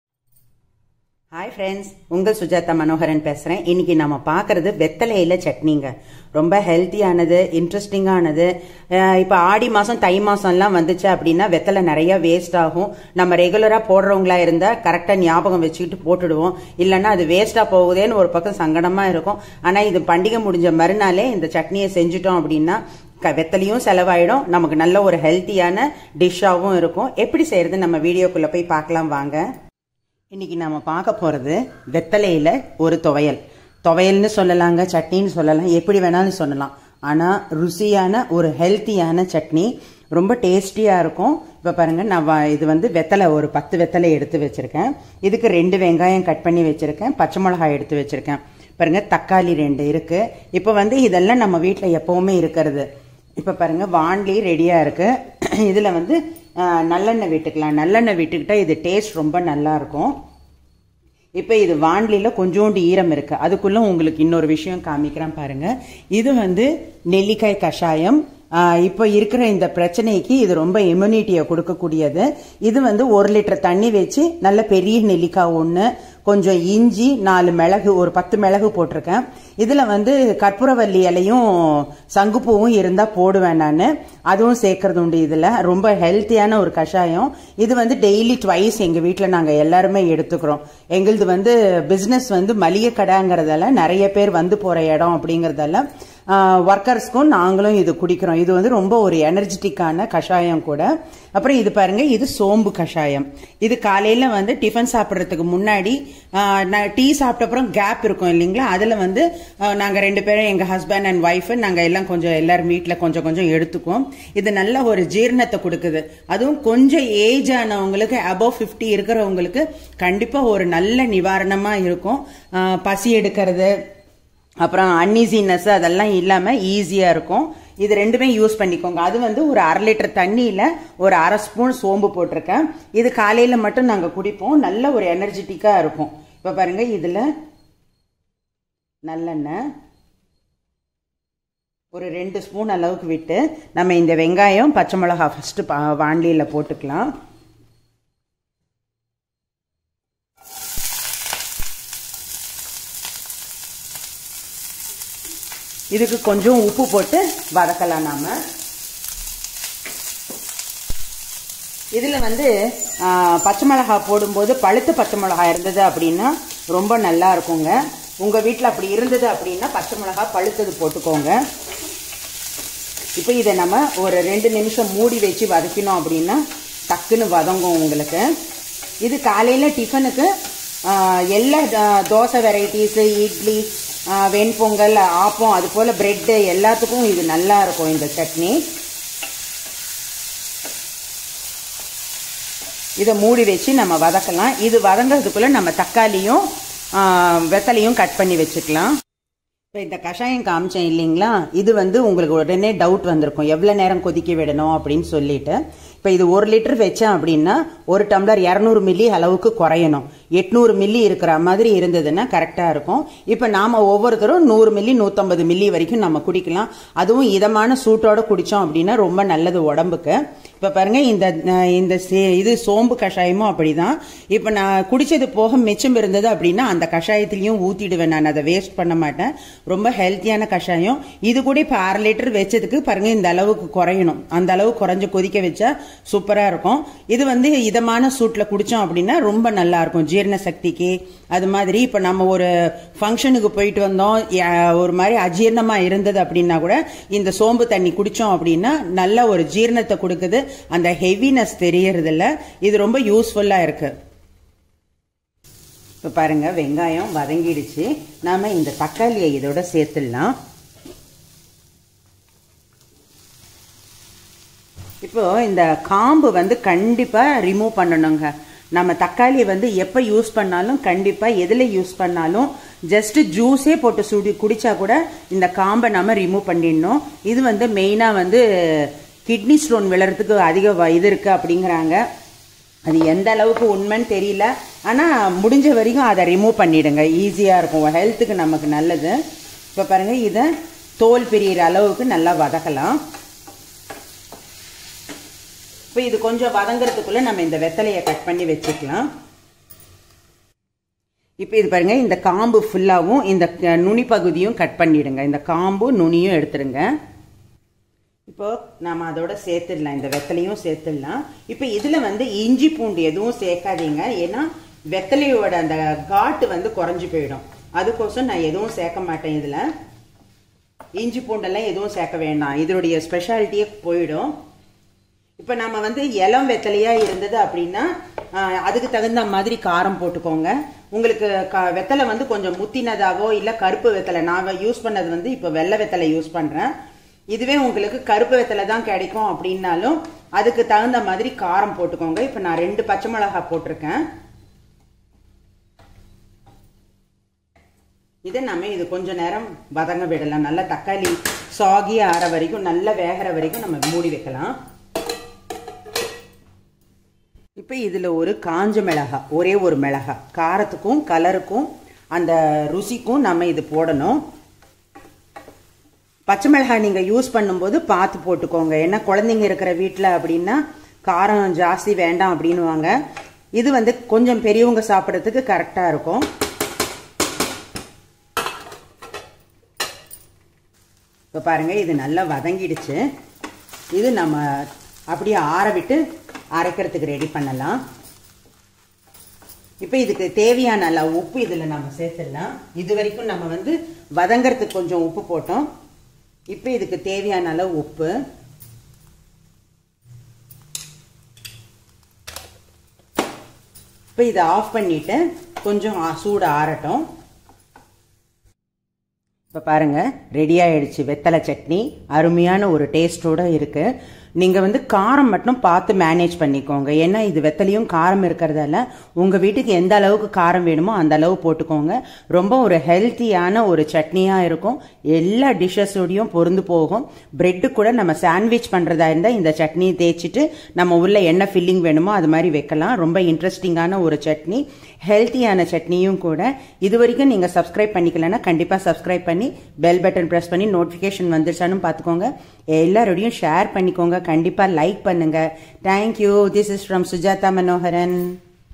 Taste & Healthy Beetle Leaves Tuvyal -- சுவையான சந்தான வெற்றிலை துவையல்
Battle Leaves 10 வெற்றிலை
Onions 2 வெங்காயம்
Tomato 2 தக்காளி
Green chillies 2 பச்சை மிளகாய்
Red chili 1 சிகப்பு மிளகாய்
Sesame oil 3 sp நல்லெண்ணெய்
Salt 1 SP உப்பு